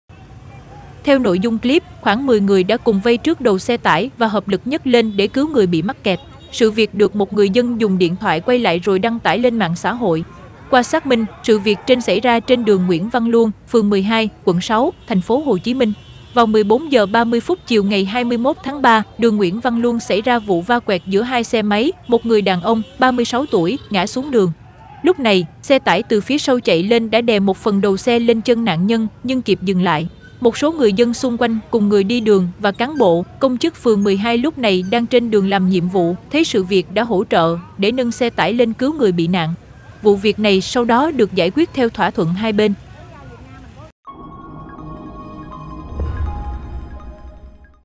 TP.HCM: Hơn chục người hợp lực nhấc đầu xe tải cứu nạn nhân bị mắc kẹt
Mạng xã hội đang lan truyền đoạn clip nức lòng với ghi chú "mỗi người phụ một tay đưa người bị nạn ra khỏi bánh xe tải" trên một đoạn đường ở TP.HCM.